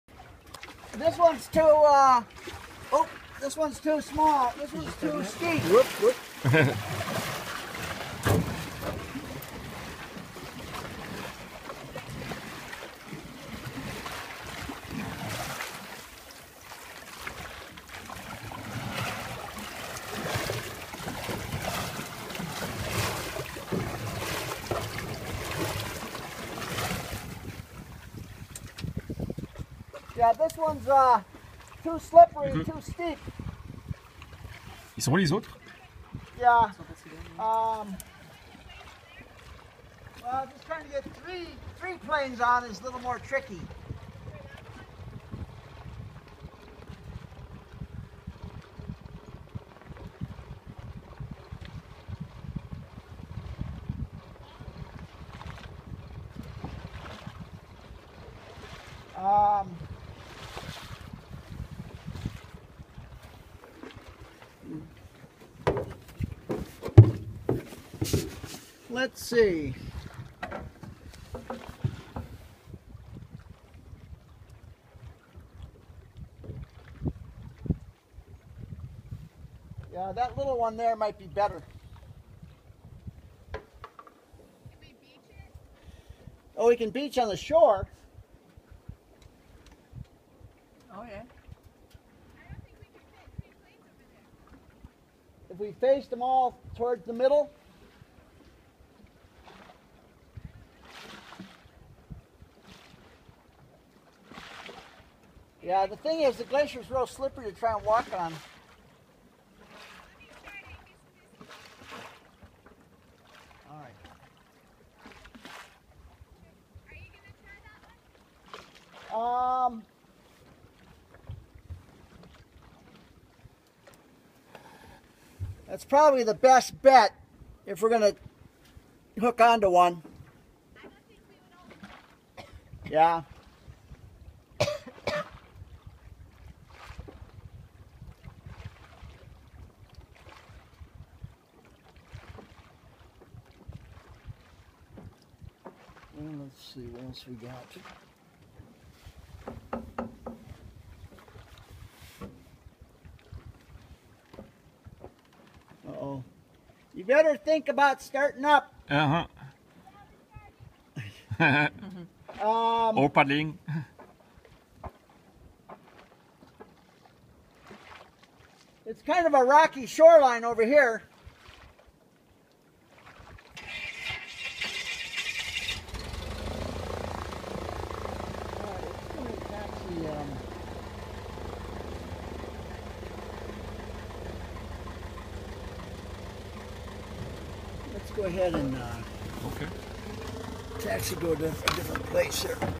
Iceberg landing in George Lake AK
Landing on a small iceberg landing in George Lake with a Cessna 185 floatplane, with Don Lee paddling.